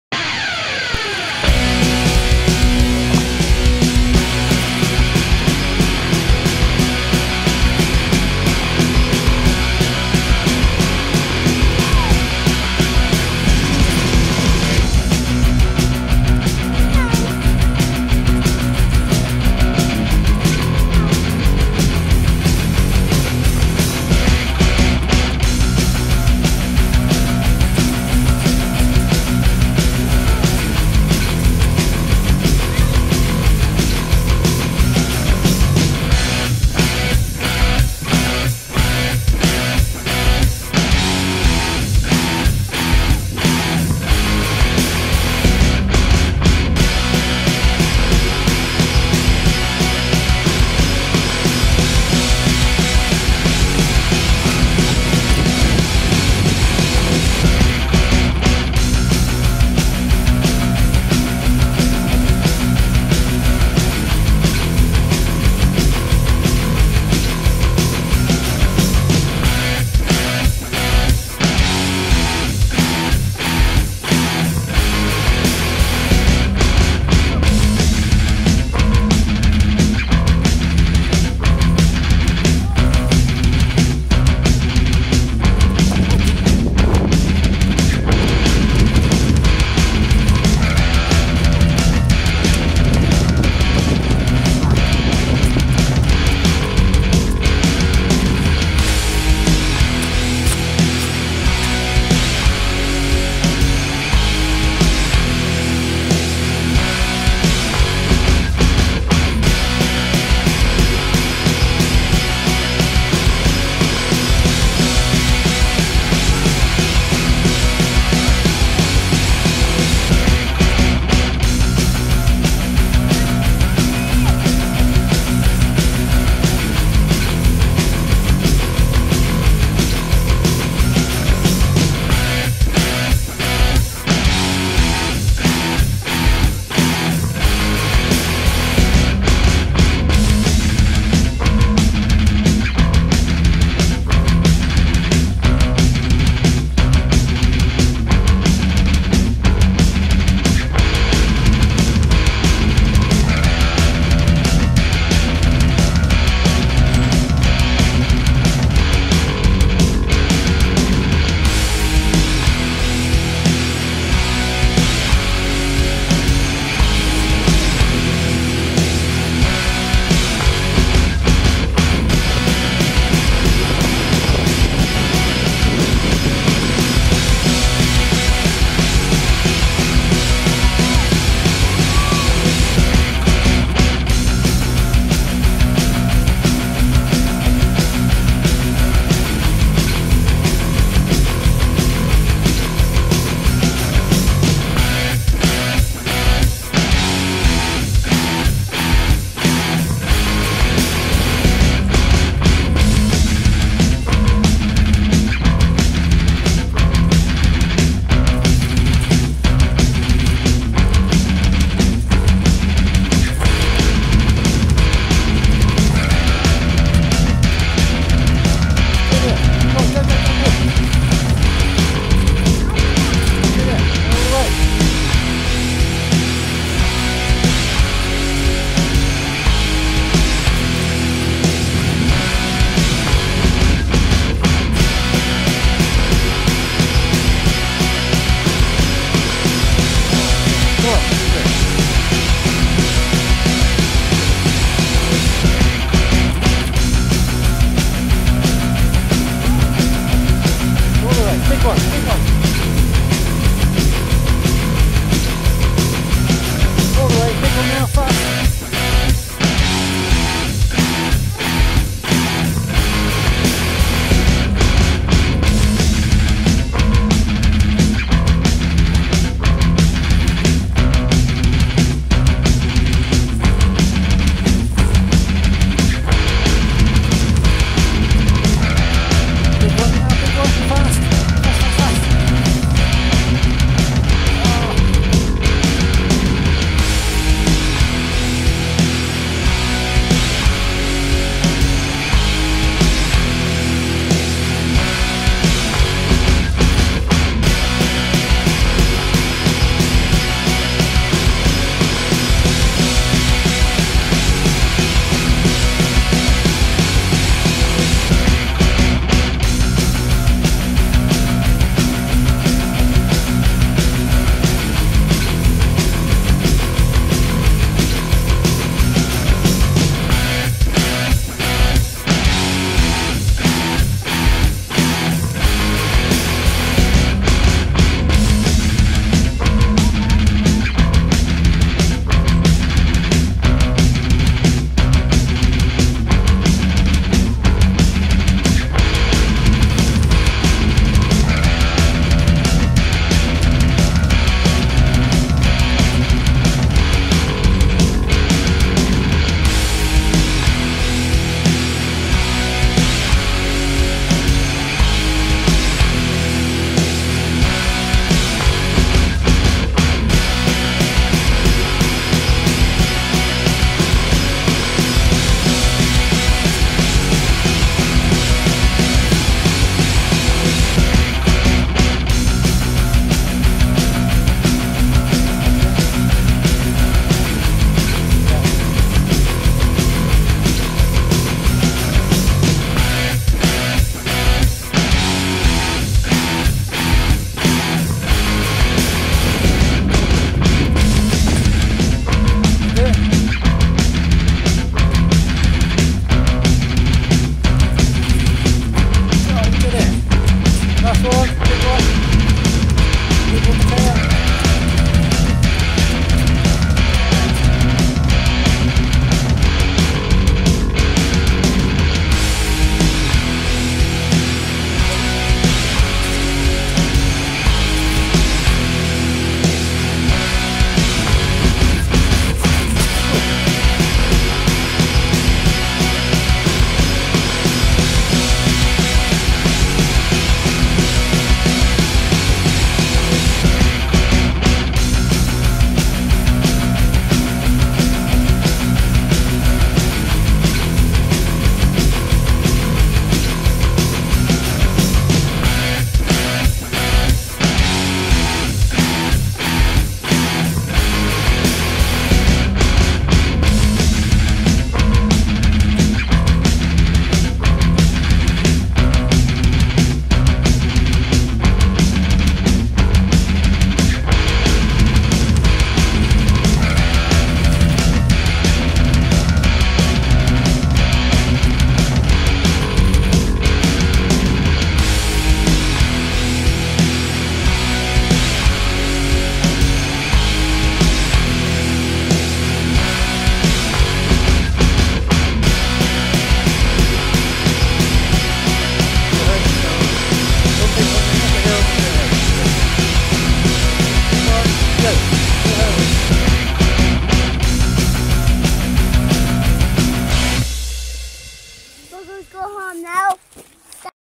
Bromham Pump Track with Rocco & Mateo and Surprise Guest "Seth!"
Seth tries to teach us, how to Pump at the Bromham Pump Track.